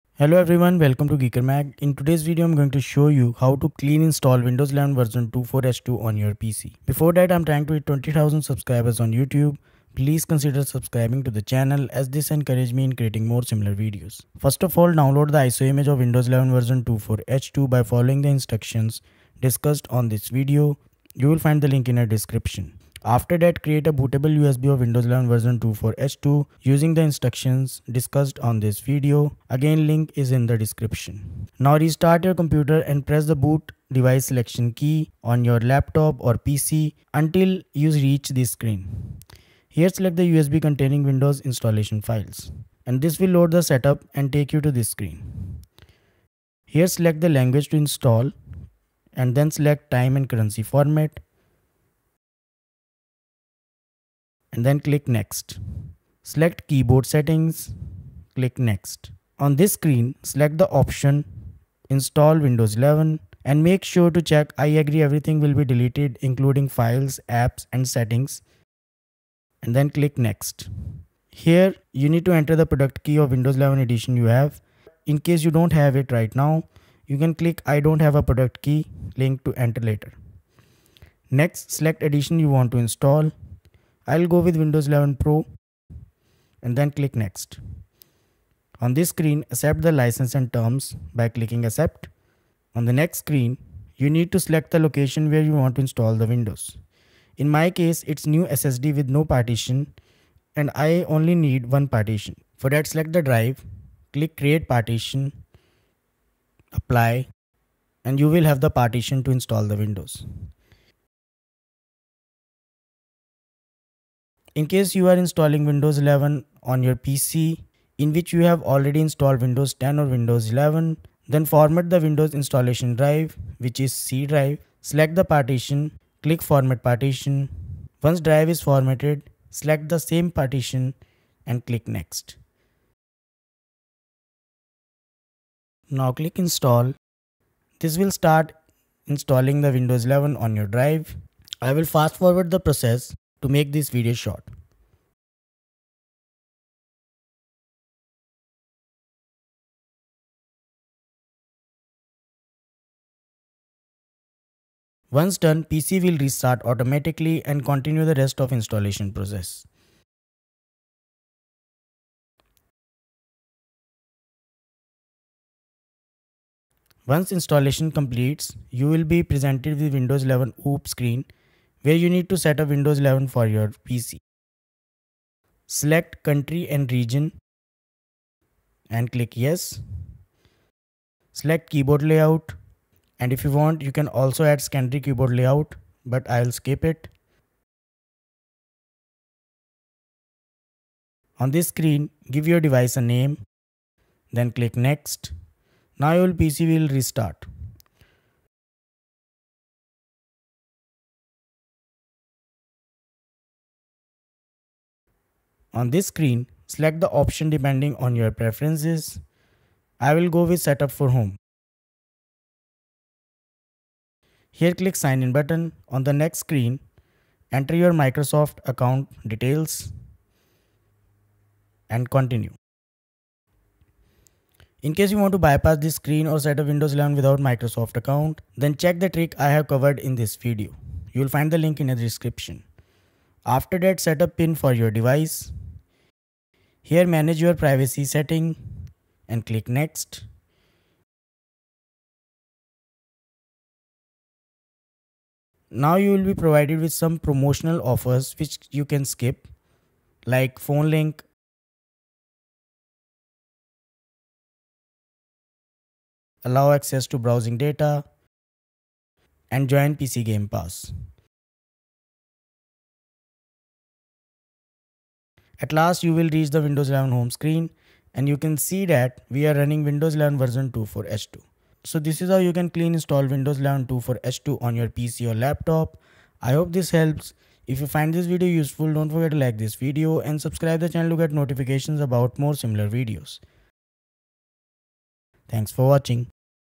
Windows 11 24H2 (Clean Install 2024) PC from USB on SSD
In this video, I'm going to show you how to clean install Windows 11 24H2 on a PC from USB on SSD

RECOMMENDED DOWNLOADS